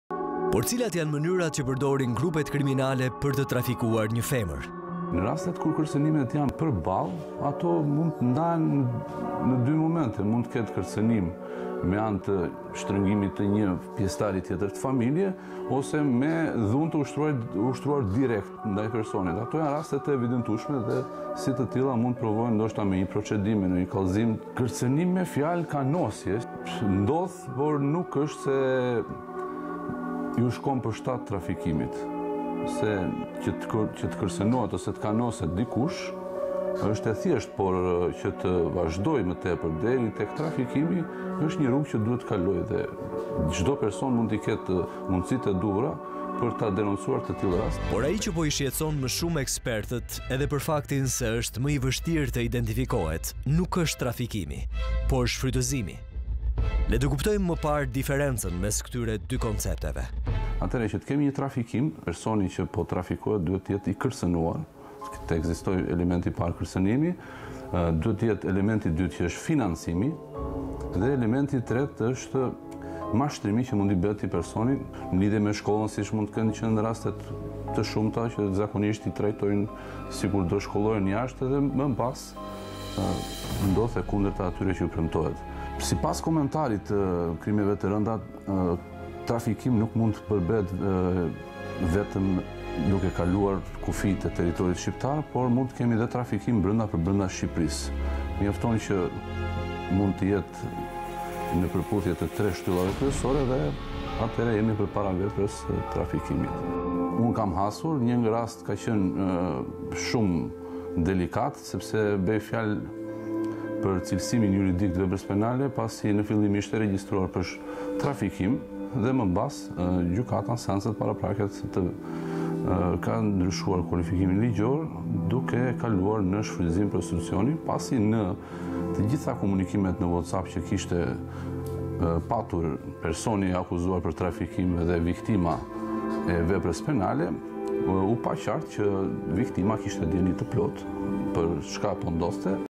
Avokati Bledar Meminaj tregon në Unpublished format e trafikimit
Avokati shpjegon me detaje si fillon trafikimi, që shkon deri në mashtrim dhe shfrytëzim.